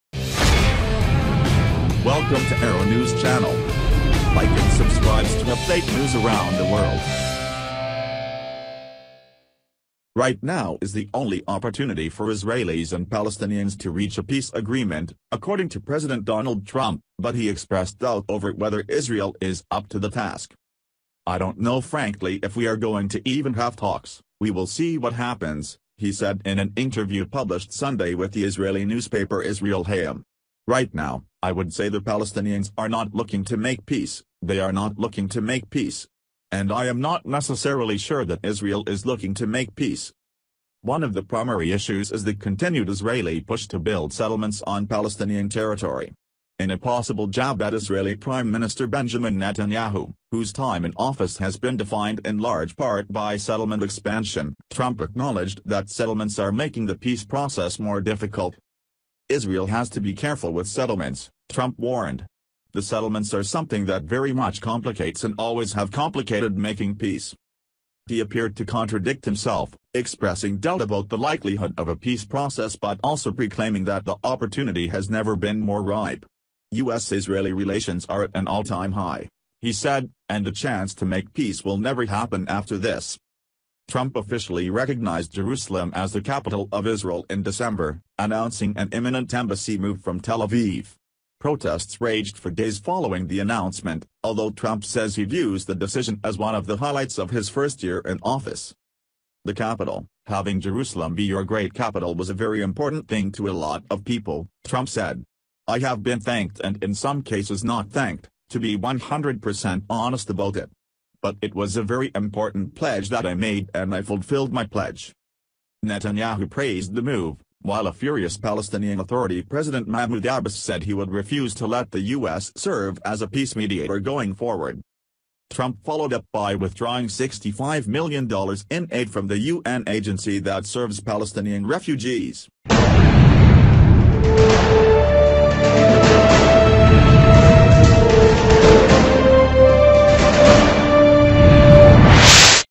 Latest news today : Trump Says Israeli Settlements Complicate Peace Process
Right now is the “only opportunity” for Israelis and Palestinians to reach a peace agreement, according to President Donald Trump ― but he expressed doubt over whether Israel is up to the task.

“I don’t know frankly if we are going to even have talks, we will see what happens,” he said in an interview published Sunday with the Israeli newspaper Israel Hayom. “Right now, I would say the Palestinians are not looking to make peace, they are not looking to make peace. And I am not necessarily sure that Israel is looking to make peace.”